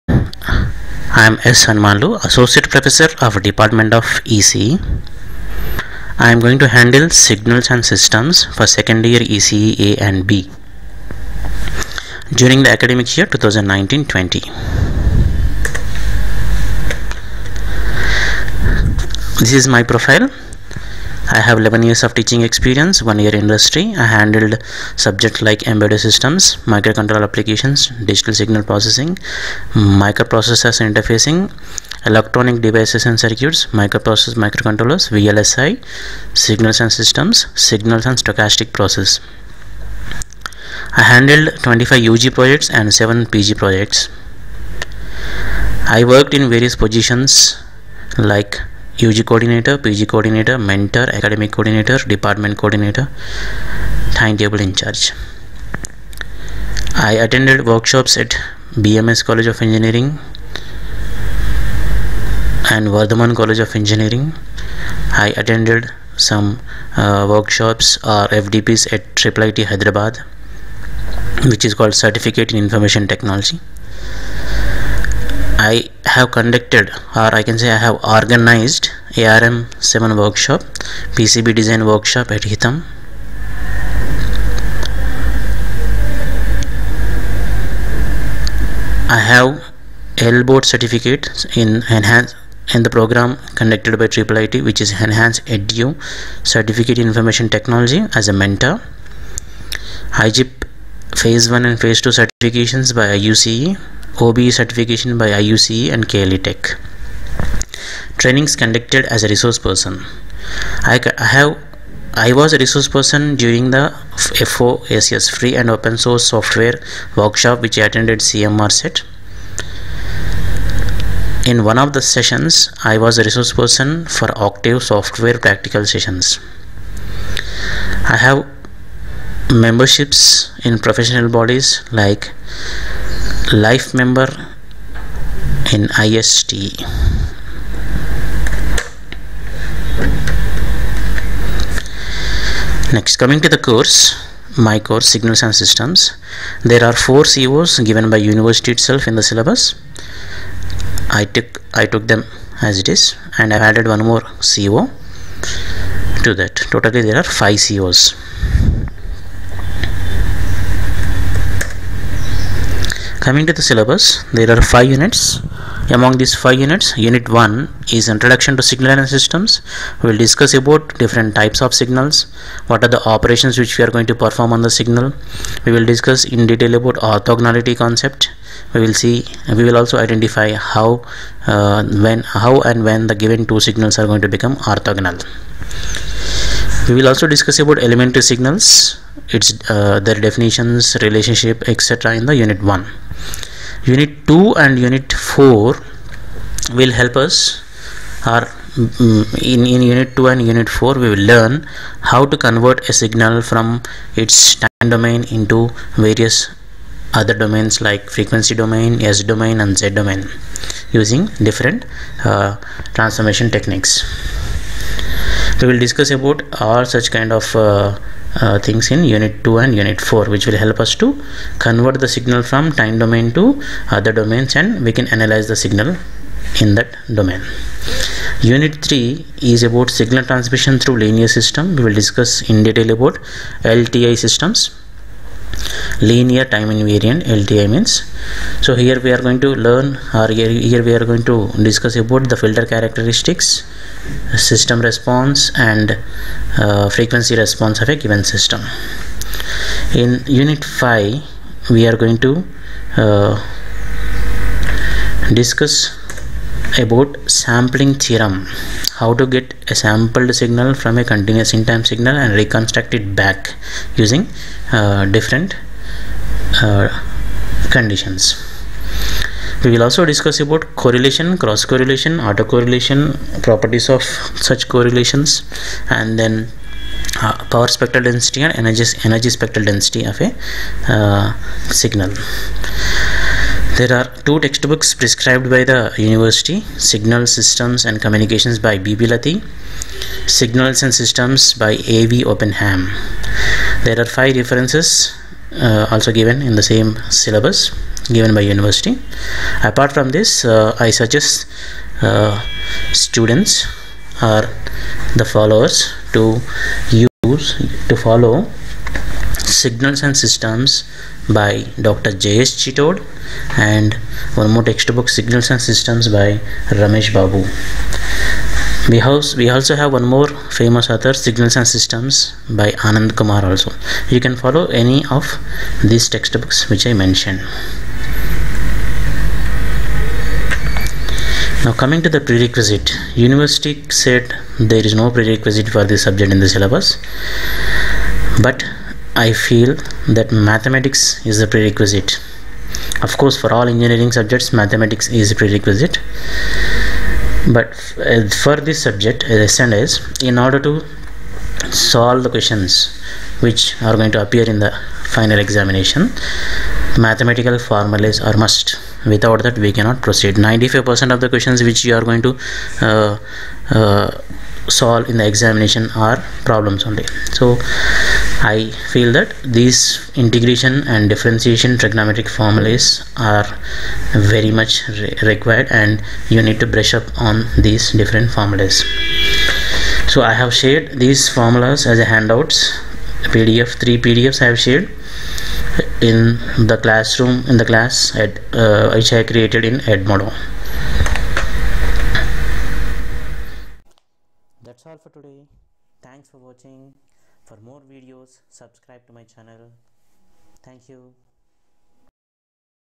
Signals and systems - Course Introduction
Teacher profile
Course outcomes
Syllabus overview
Text books
Reference books
Prerequisites to course  are available in this video